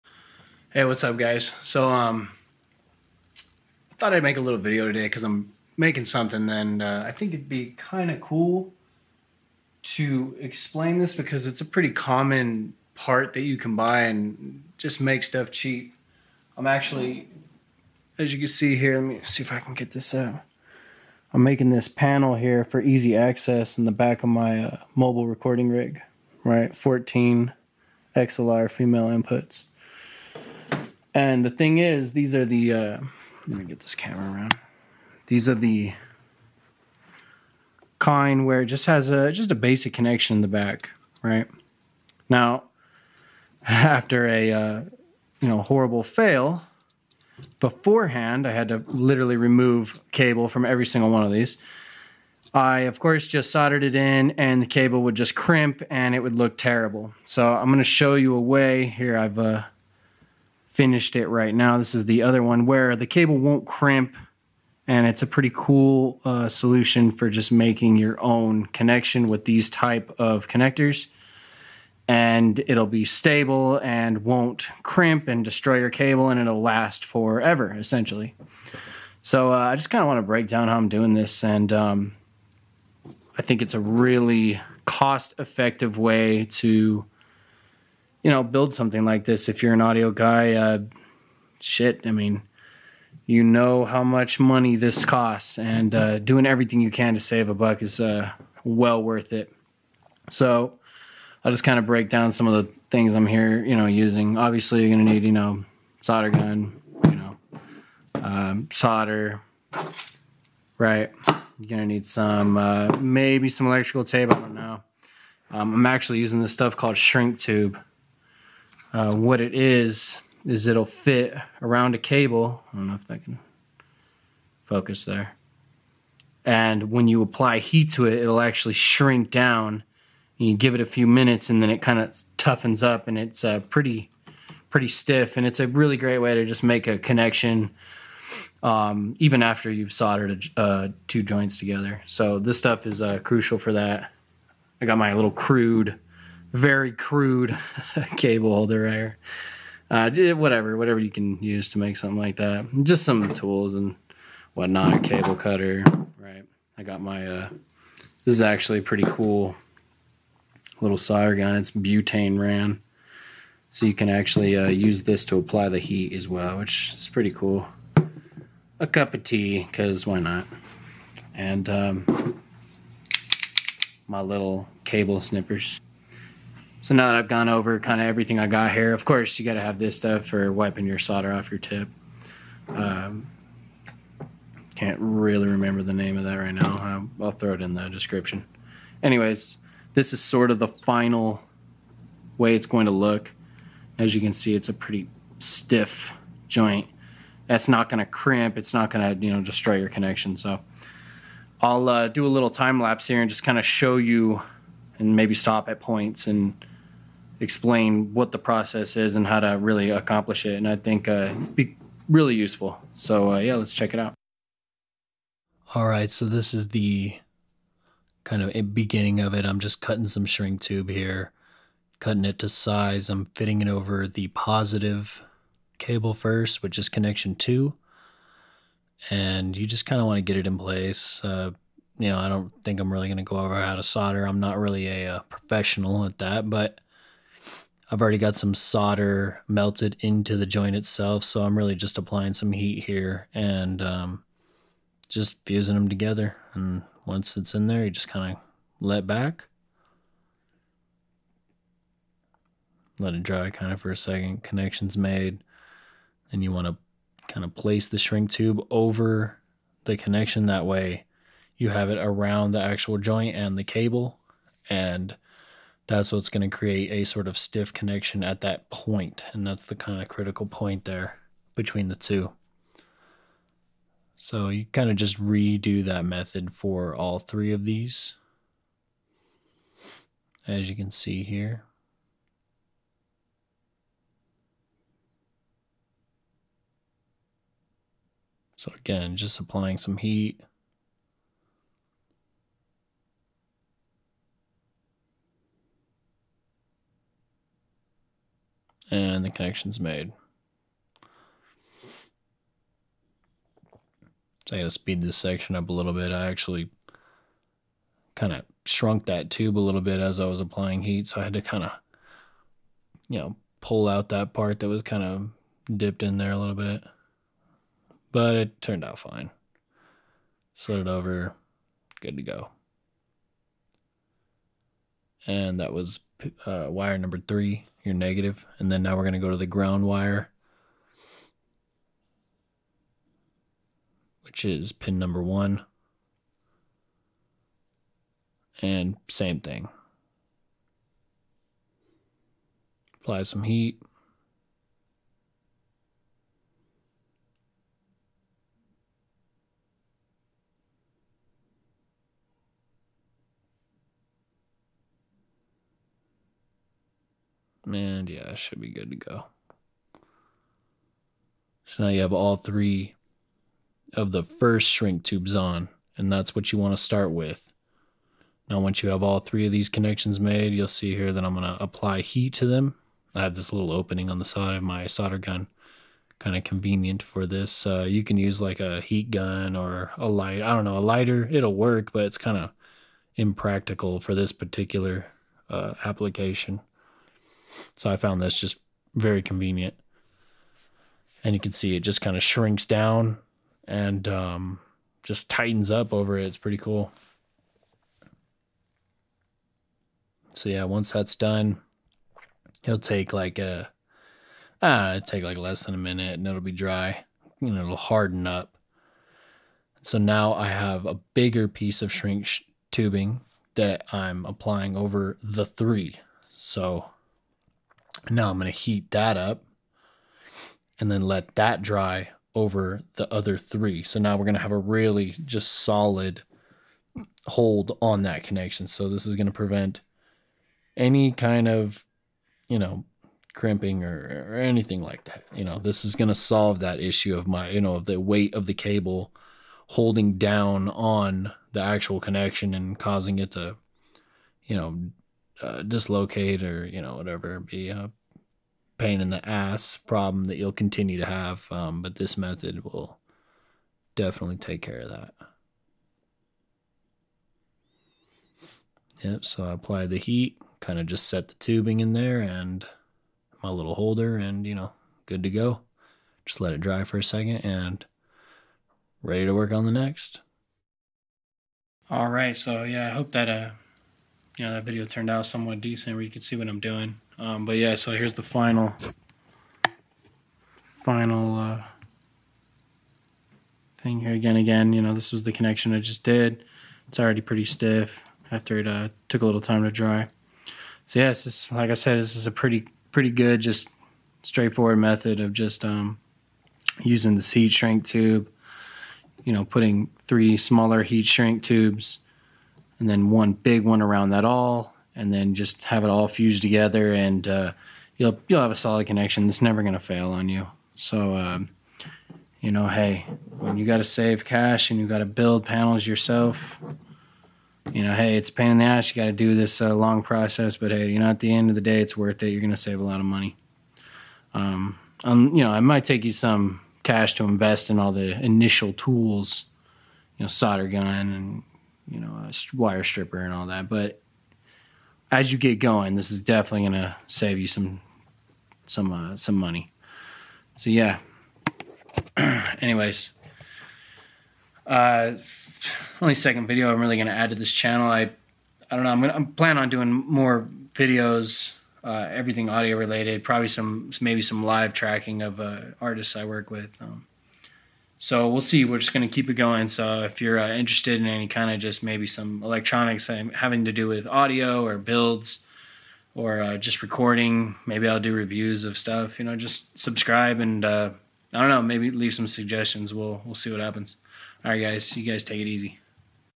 XLR panel build ( How to secure the connection with shrink tube)
Here I show you a cool way of securing your soldered XLR cable to the XLR female connector without having the weight of the cable cause any crimping or damage to the connection by using shrink tubing. and of corse this method would work for any connection you are making be it XLR, TRS..etc